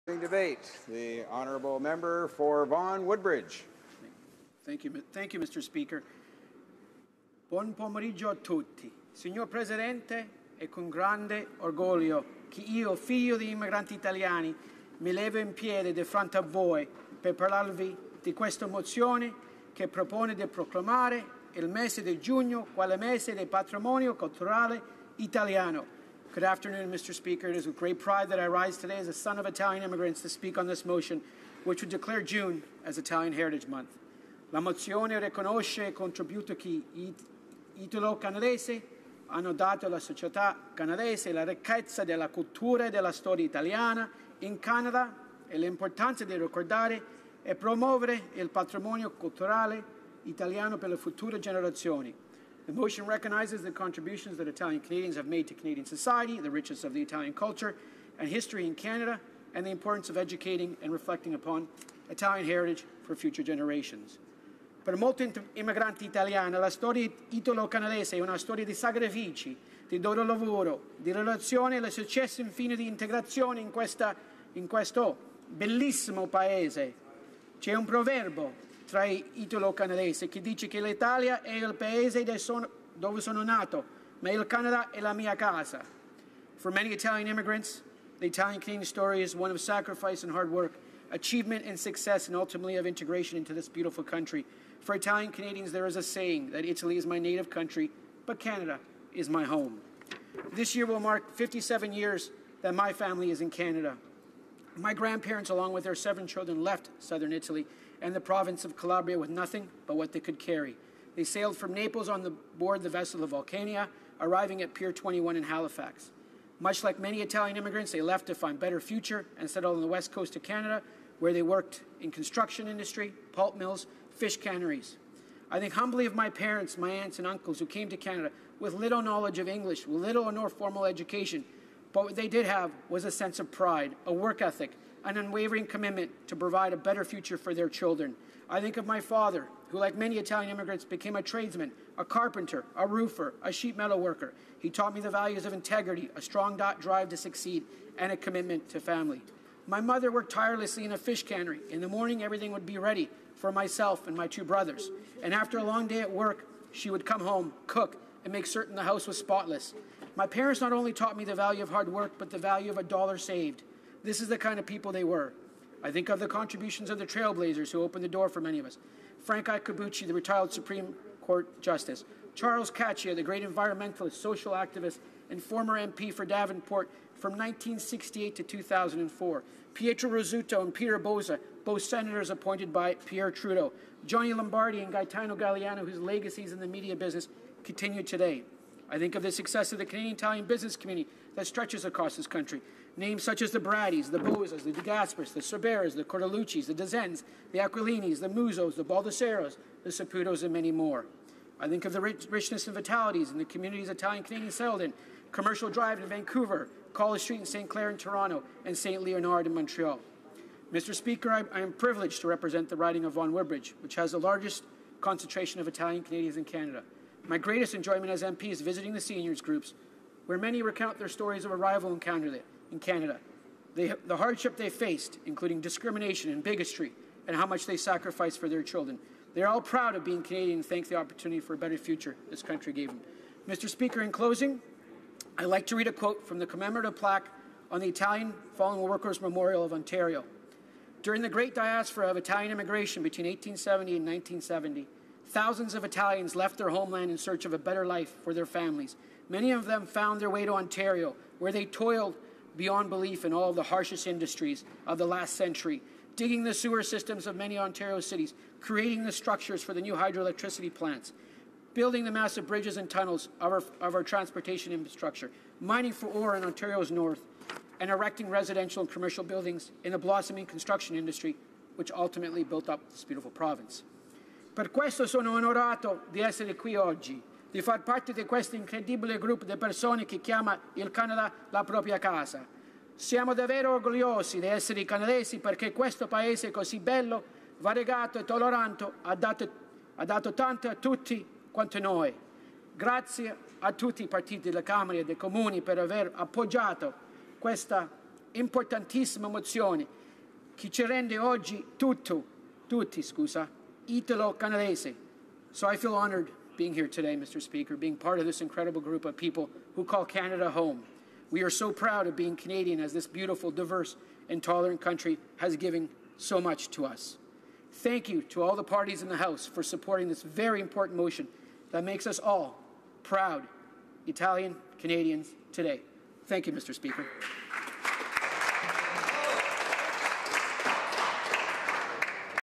Italian Heritage
MP Francesco Sorbara speaks in the House of Commons on Motion M-64 in support of Italian Heritage Month.